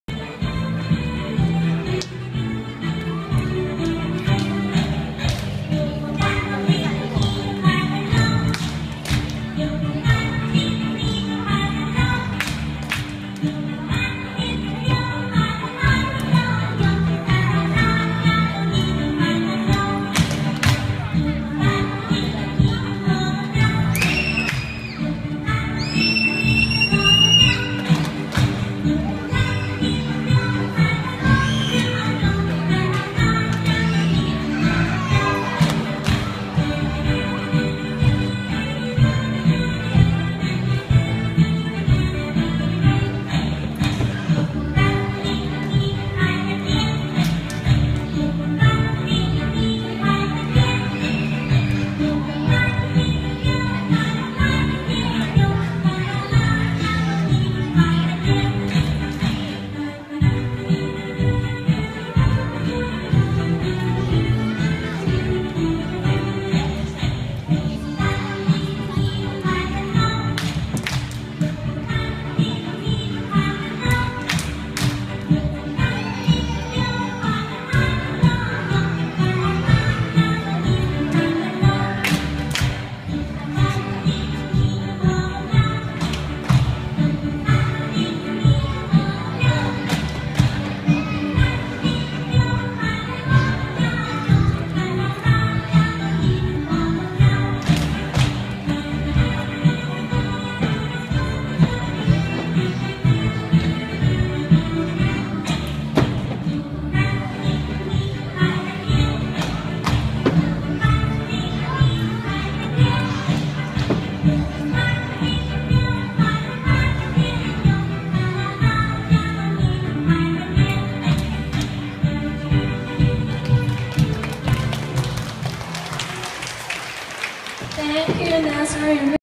Glar's performance for Chinese New Year 2024. (2.2yrs old)
Glar performed dance at Wells international with his classmate for Chinese New Year 2014. We had so much fun watching him standing still while his friends were dancing with joy!
28/1/14